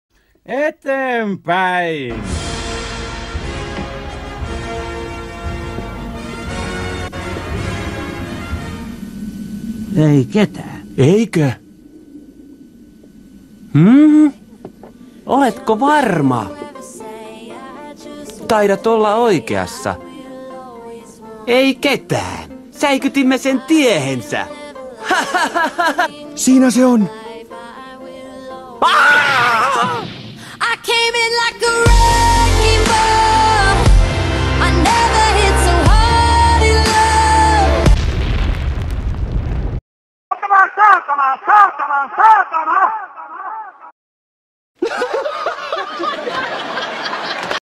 Koronavirus leviää Muumilaaksoon!
Breaking! Muumitalo lukitaan yöksi koronaviruksen takia